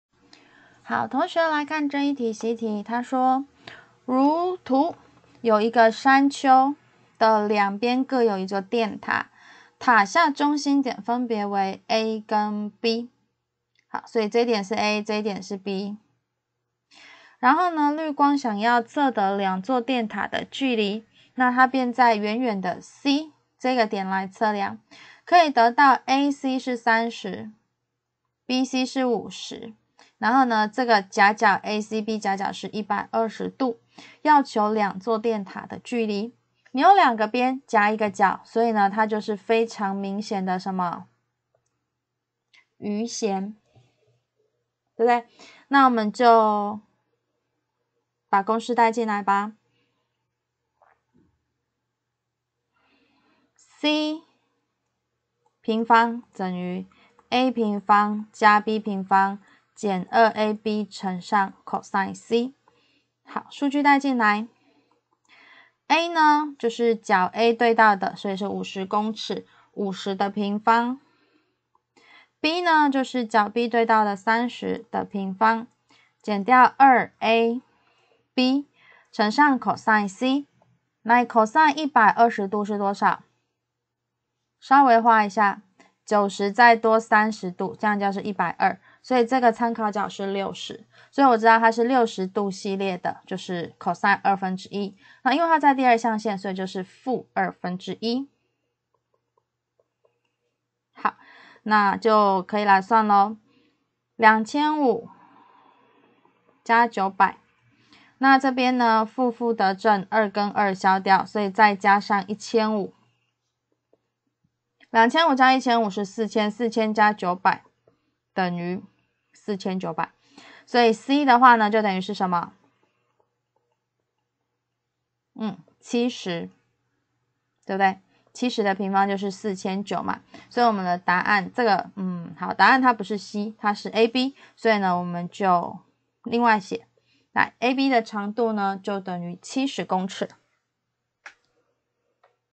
技高數學B（四）乙版Unit1習題第2題 泰宇出版．翔宇文化
由周秉樺老師精心說明，請參考技高數學B（四）乙版課本